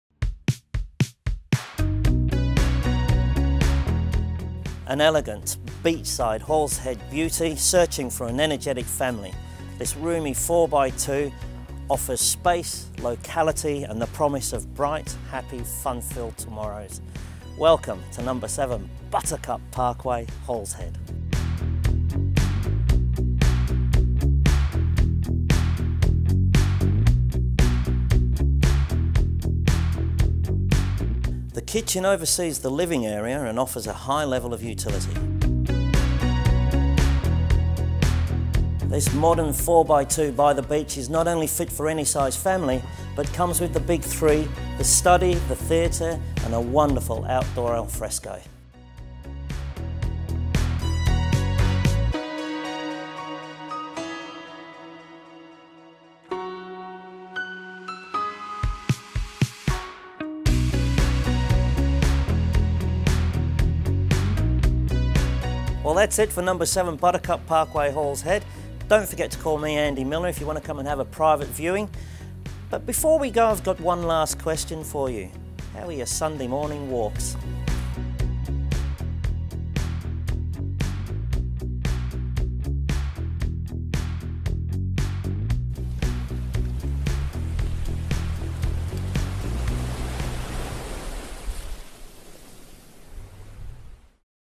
Andy Miller introduces 7 Buttercup Parkway, Halls Head
It's the possibility of having a dream come true that makes life a truly wonderful thing. Through the eyes of your children's lives memories unfold and are delivered in your later years as sweet remembrances and finely perfumed halcyon recollections. Great homes are a great foundation for the greatest of memories. As the kids fly out the front door, bodyboard under one arm, towel in the other, over the road to throw themselves at an oceanic playground full of adventure and natural wonder, you can take solace that these moments are the building blocks of a wonderful life.

Less than 150metres to the water this 230sqm home sprawls back from its neatly landscaped Buttercup Parkway frontage in a welcoming, friendly sweep. The front door and hallway are in double proportion to a standard home.

The main bedroom is isolated at the front of the home for privacy and peace. The room is king-sized and has a smart, modern ensuite with separate toilet and spacious shower enclave.

As you pass the lighted presentation wall along the winding hall you come across a great office space, ready for the fit-out that suits your own personal business or studies.

The main living areas open up like a secret oasis, offering more than enough family space overseen by the commanding and highly functional kitchen. Behind the kitchen lies the theatre room. Fully set up for any audio-visual gear you care to lug in. If you crack the windows you can hear the waves so this room should be relegated to winter only!

The extra bedrooms are down the rear corridor in a separate wing accentuating once again the two P's (Peace and Privacy). The fourth bedroom can also be used as a games room which further expands this home's multiplicity as an all-round family winner. Back here is where the Alfresco sits with a super large timber deck and the lure of a billion BBQ's

Things to smile about:
• Beachside bliss only 150mtrs away
• Side access to boat or caravan
• Super low maintenance grounds
• R/C A/C split system
• Office/theatre/alfresco
• Freshly painted throughout
• Large 4x2 with the lot

Please allow me to turn your dreams into the here and now call Andy Miller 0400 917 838